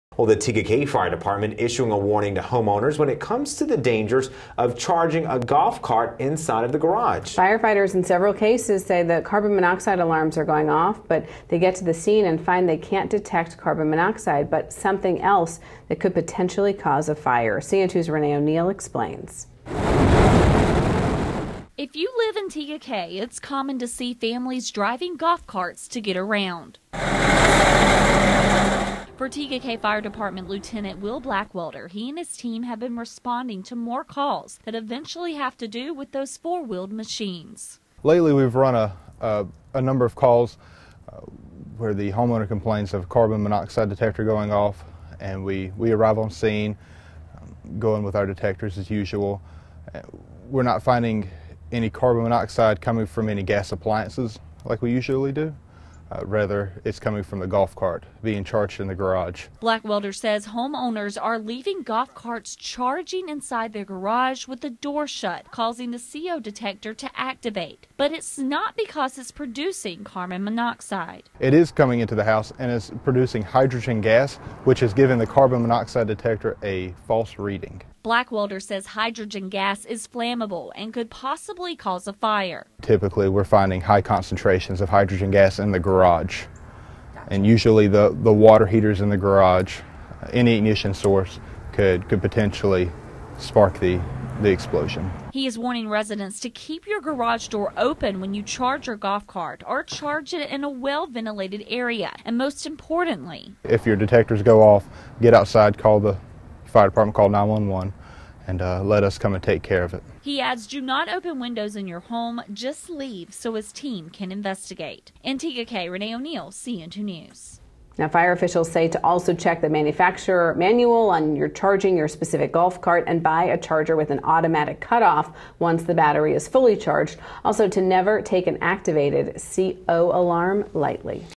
Warning to Residents
The Tega Cay Fire Department issuing a warning to homeowners when it comes to the dangers of charging your golf cart inside the garage.